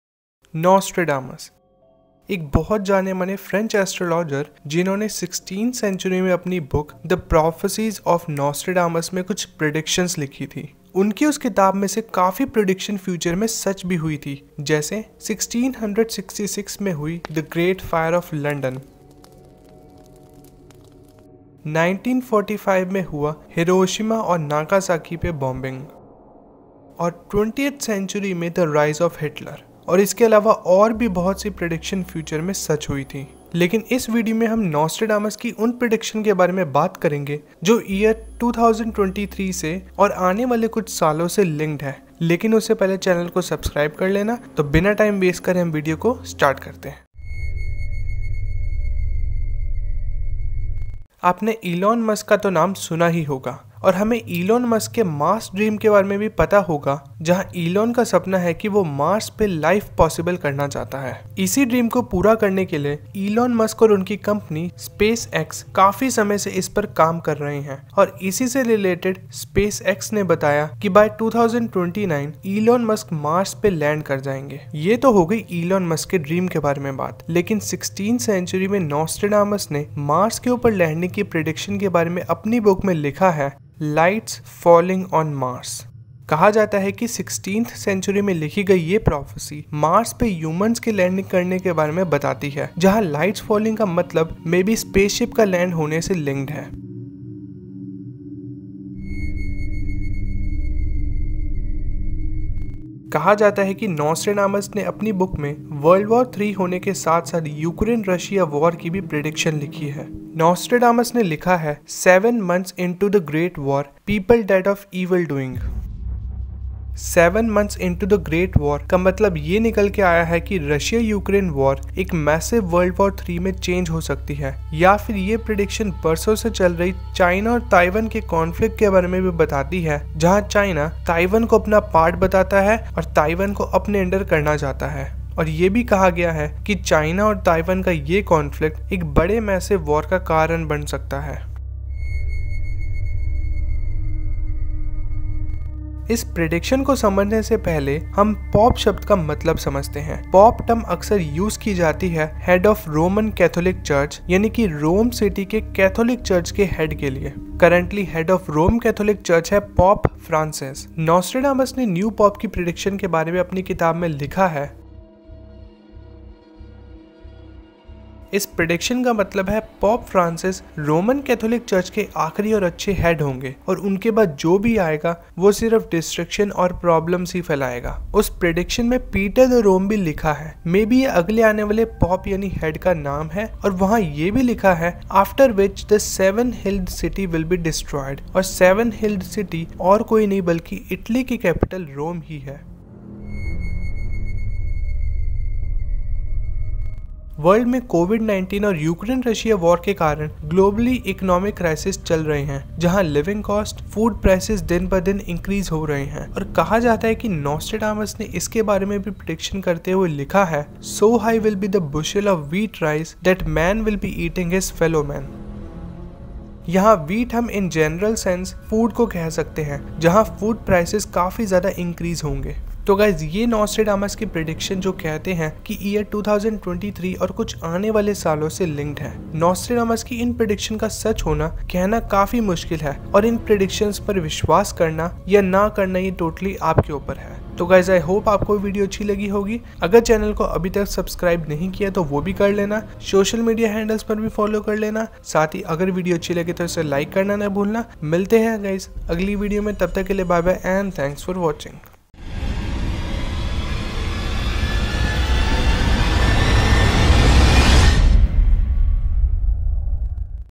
World War 3 ? | Nostradamus Predictions 2023 (Hindi) | Ghostly Tube
There are some Nostradamus predictions that are related to year 2023 and some upcoming years.

0:00 - Intro
0:47 - Prediction #1 Disaster of Mars Mission
1:38 - Prediction #2 World War 3
2:21 - Prediction #3 New Pope Prediction
3:13 - Prediction #4 Global Economic Crisis